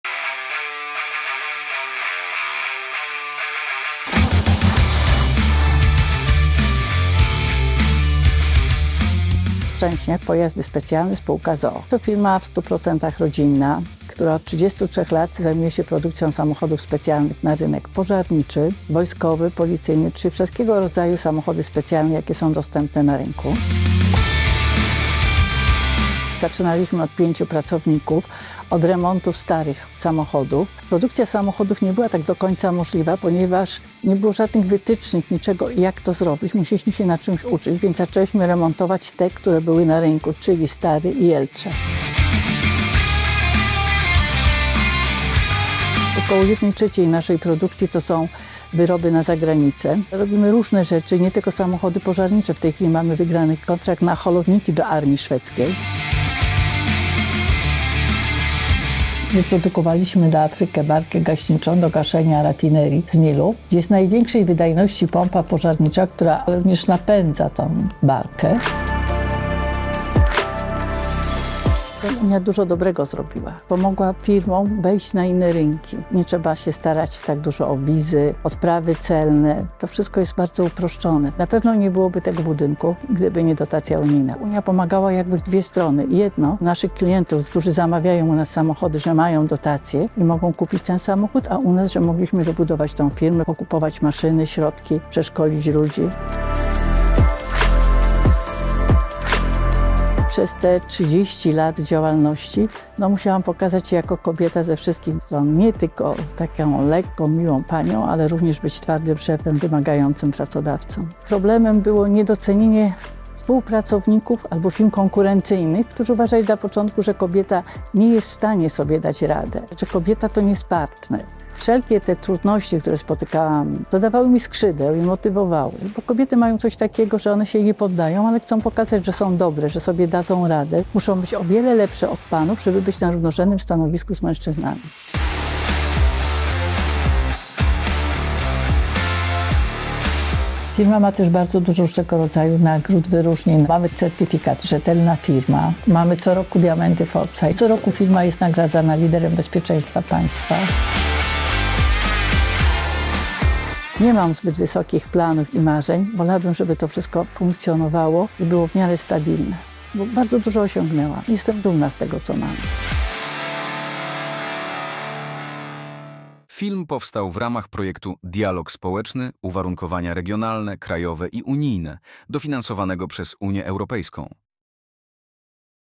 Szczęśniak Pojazdy Specjalne – polska technologia w światowej lidze
Rodzinna firma z Bielska-Białej, która projektuje i produkuje pojazdy ratownicze, wojskowe i specjalistyczne dla klientów z 35 krajów. Dzięki innowacjom i inwestycjom wspieranym przez Unię Europejską stała się jednym z liderów swojej branży.

Film powstał w ramach projektu pt. „Dialog społeczny: uwarunkowania regionalne, krajowe i unijne”. Projekt jest dofinansowany przez Unię Europejską w ramach programu FERS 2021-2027.